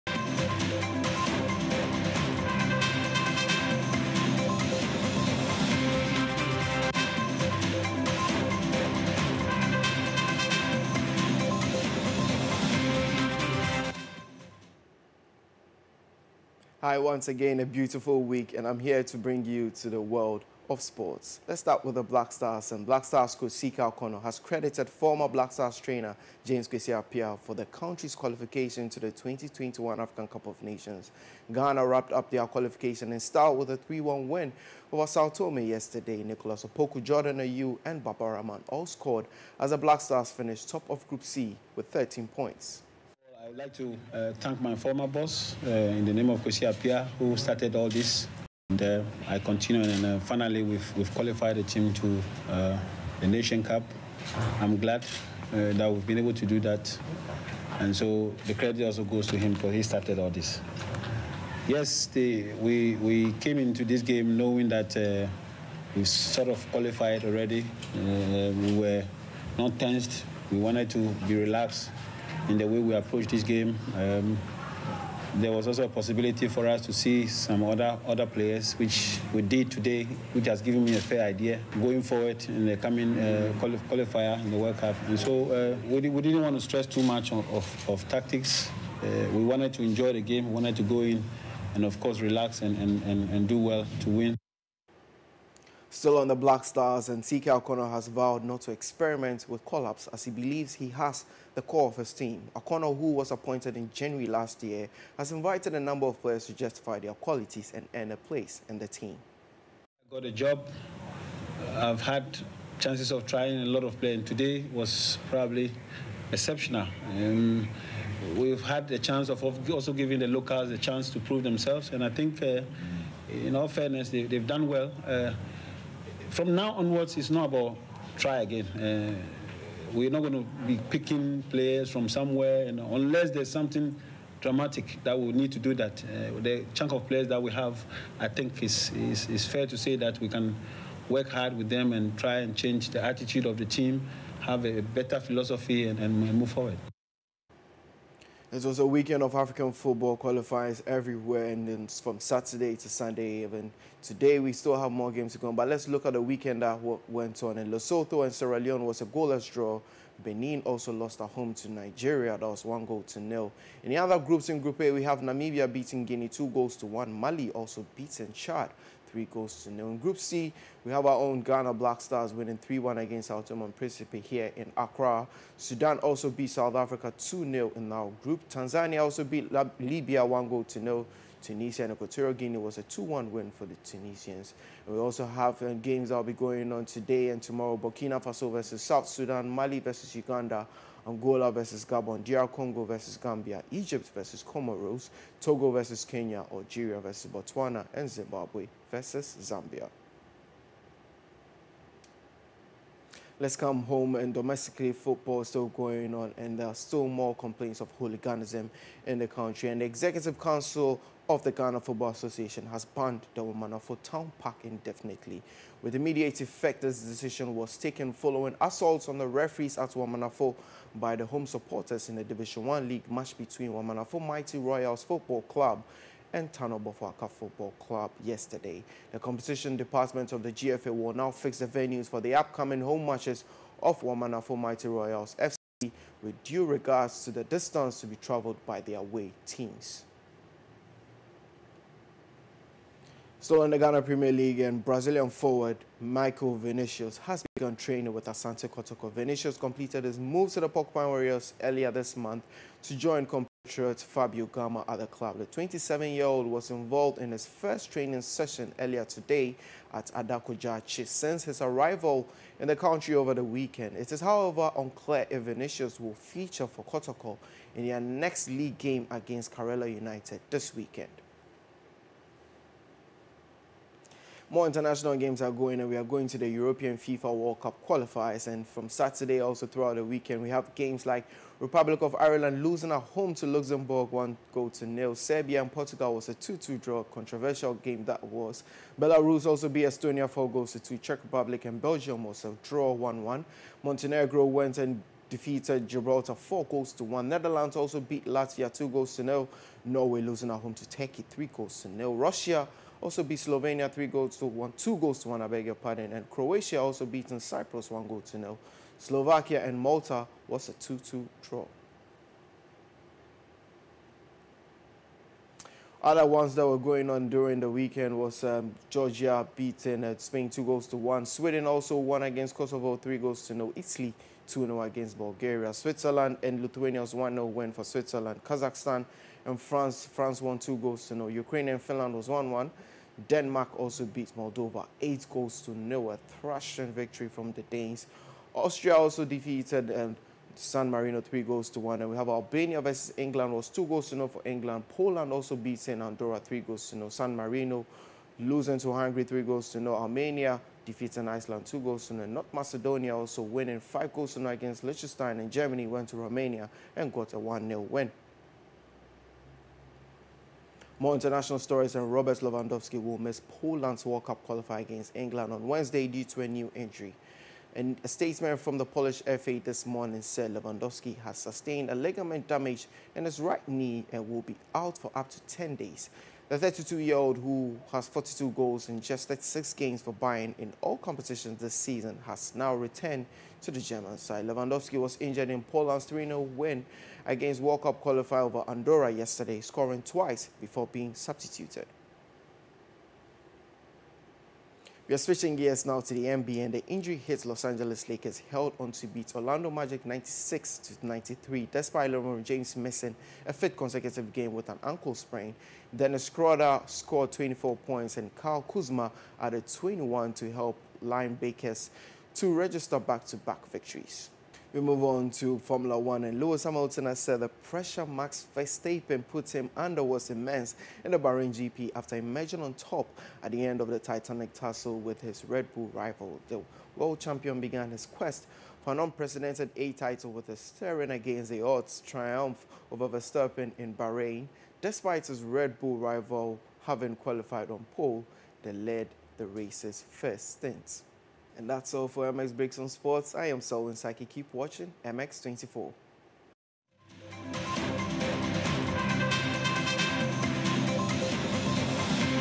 MX BREAKS: SPORTS- CK Akonnor credits Kwasi Appiah for Ghana's Qualification
MX Breaks brings you the latest news headlines in Ghana and across the globe!
In this edition:

- CK Akonnor credits Kwasi Appiah for Ghana's Qualification
- CK Akonnor vows not to experiment with Black Stars call-up again
- Wamanafo Town park banned indefinitely by GFA
- Michael Vinicius begins training with Asante Kotoko